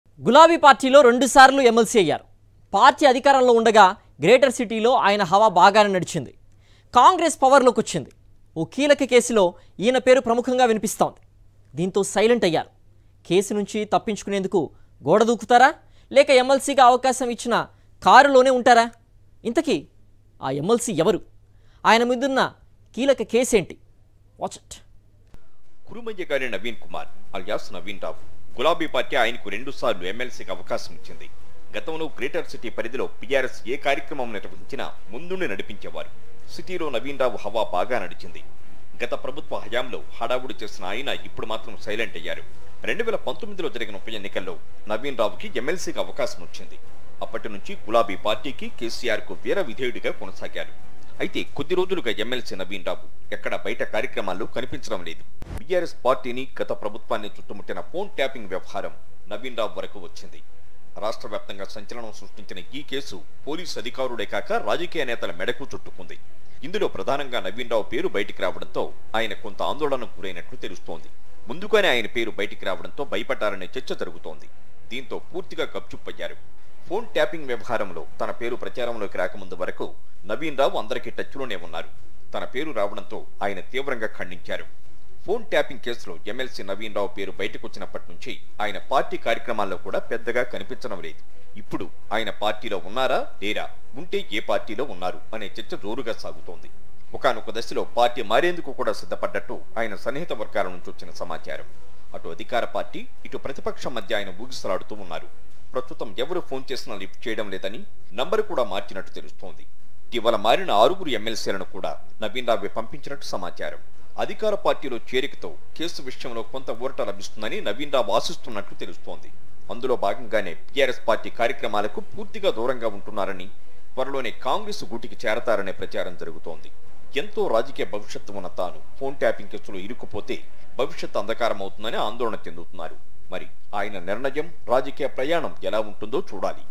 Political Corridor: Will BRS MLC Naveen Rao Joins In Congress | Phone Tapping Case | KCR vs Revanth
Political Corridor: Will BRS MLC Naveen Rao Joins In Congress | Phone Tapping Case | KCR vs Revanth @SakshiTV